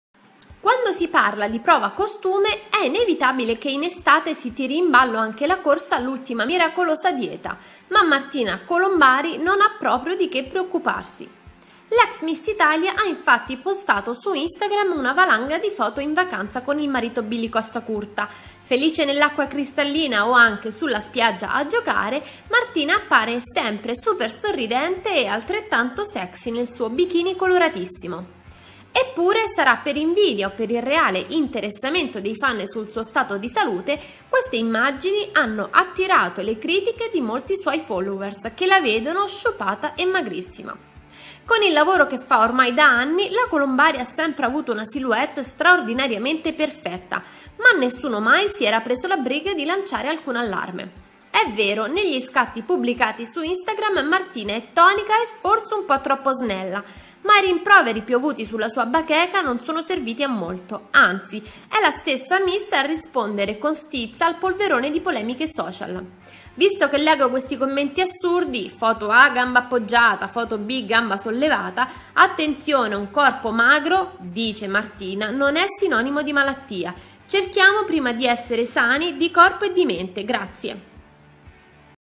Martina Colombari, critiche social: troppo magra
La modella risponde stizzita su Instagram